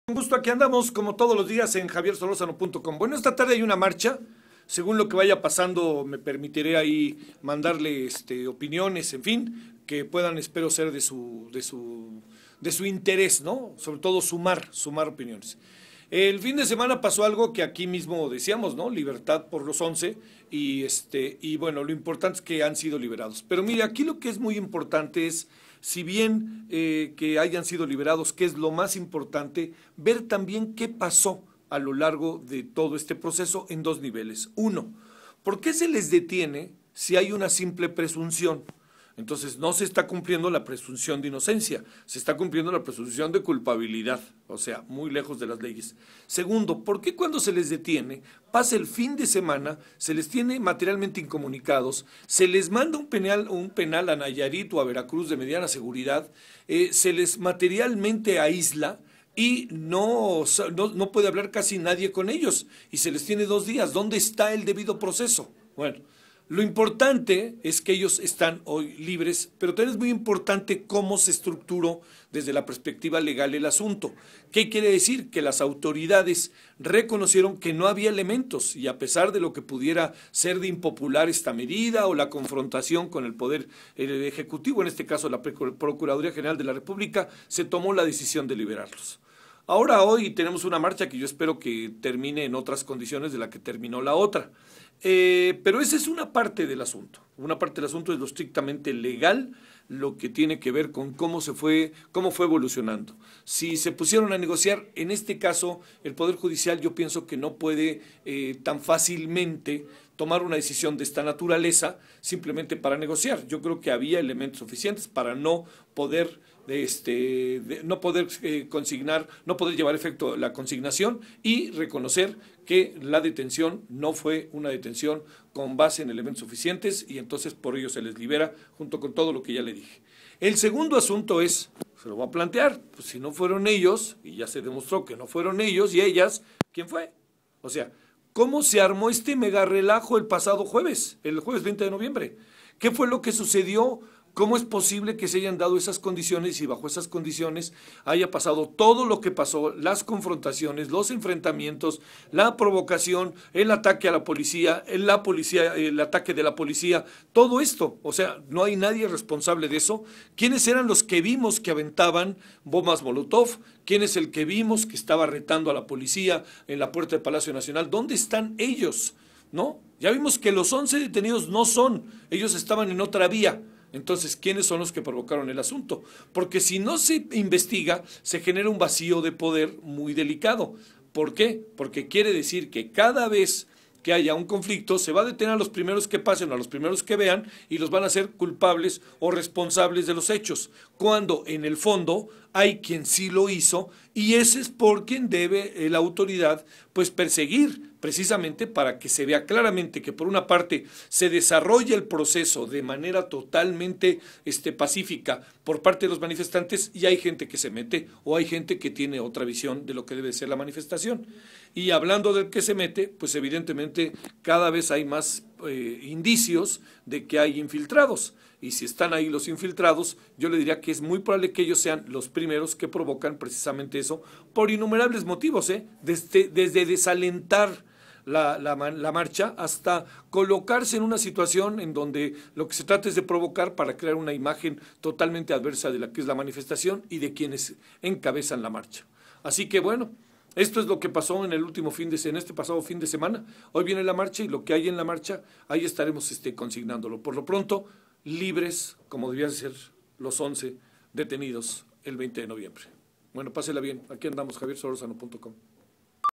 En libertad...
Se debe de analizar lo sucedido con los once detenidos, durante la marcha del 20 de nov, ya que, la misma autoridad reconoció que no existen razones para su arresto, y por ello se les dejo en libertad, esto nos habla de un proceso mal llevado.